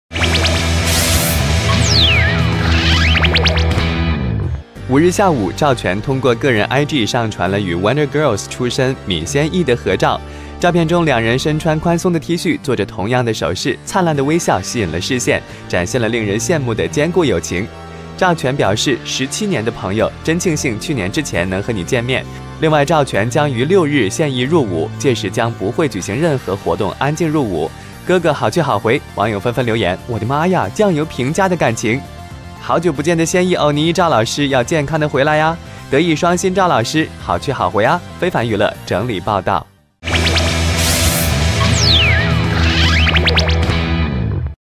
17年至親認證！趙權入伍前夕曬與先藝合照
17年至親認證！趙權入伍前夕曬與先藝合照